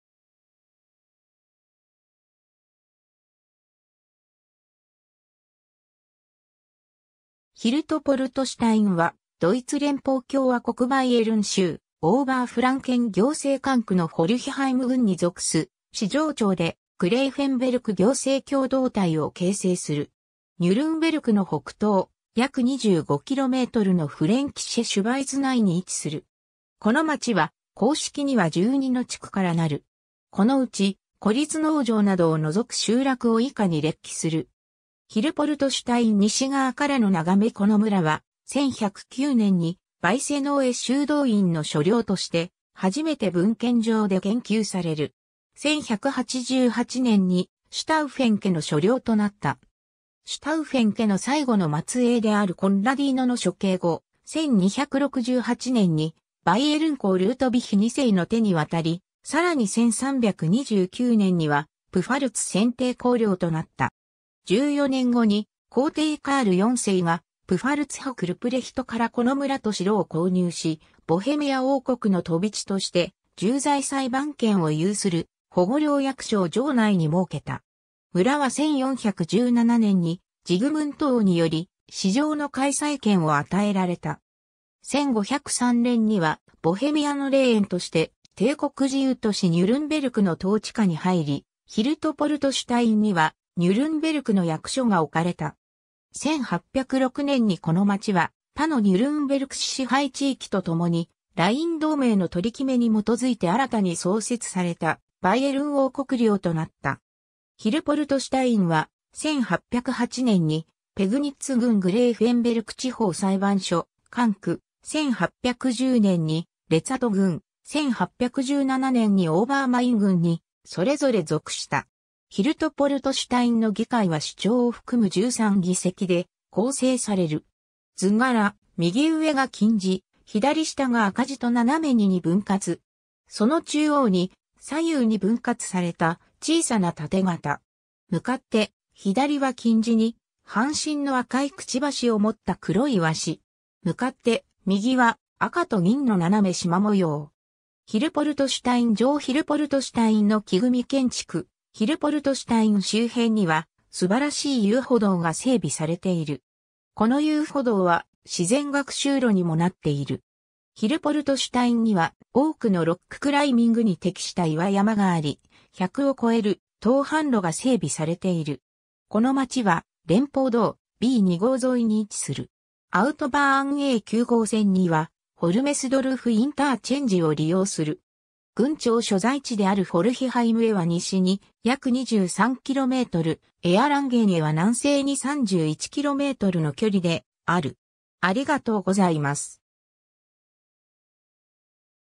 ヒルトポルトシュタイン
ヒルトポルトシュタイン (ドイツ語: Hiltpoltstein) は、ドイツ連邦共和国バイエルン州オーバーフランケン行政管区のフォルヒハイム郡に属す市場町で、グレーフェンベルク行政共同体を形成する。
ニュルンベルクの北東約25kmのフレンキシェ・シュヴァイツ内に位置する。
 この町は、公式には12の地区 (Ort) からなる。
 ヒルポルトシュタイン西側からの眺め この村は、1109年にヴァイセノーエ修道院の所領として、初めて文献上で言及される。
1188年にシュタウフェン家の所領となった。
シュタウフェン家の最後の末裔であるコッラディーノの処刑後、1268年にバイエルン公ルートヴィヒ2世の手に渡り、さらに1329年にはプファルツ選帝侯領となった。
14年後に皇帝カール4世がプファルツ伯ルプレヒトからこの村と城を購入し、ボヘミア王国の飛び地として重罪裁判権を有する保護領役所を城内に設けた。
村は1417年にジグムント王により市場の開催権を与えられた。
1503年にはボヘミアのレーエンとして帝国自由都市ニュルンベルクの統治下に入り、ヒルトポルトシュタインにはニュルンベルクの役所が置かれた。
1806年にこの町は、他のニュルンベルク市支配地域とともに、ライン同盟の取り決めに基づいて新たに創設されたバイエルン王国領となった。
ヒルポルトシュタインは、1808年にペグニッツ郡グレーフェンベルク地方裁判所管区、1810年にレツァト郡、1817年にオーバーマイン郡にそれぞれ属した。
 ヒルトポルトシュタインの議会は首長を含む13議席で構成される。
 図柄：右上が金地、左下が赤地と斜めに二分割。
その中央に左右二分割された小さな盾型。
向かって左は金地に半身の赤い嘴を持った黒い鷲。
向かって右は赤と銀の斜め縞模様。
 ヒルポルトシュタイン城 ヒルポルトシュタインの木組み建築 ヒルポルトシュタイン周辺には、すばらしい遊歩道が整備されている。
この遊歩道は、自然学習路にもなっている。
ヒルポルトシュタインには多くのロッククライミングに適した岩山があり、100を超える登攀路が整備されている。
 この町は、連邦道B2号沿いに位置する。
アウトバーンA9号線にはホルメスドルフ・インターチェンジ（約9km離れている）を利用する。
郡庁所在地であるフォルヒハイムへは西に約23km、エアランゲンへは南西に31kmの距離である。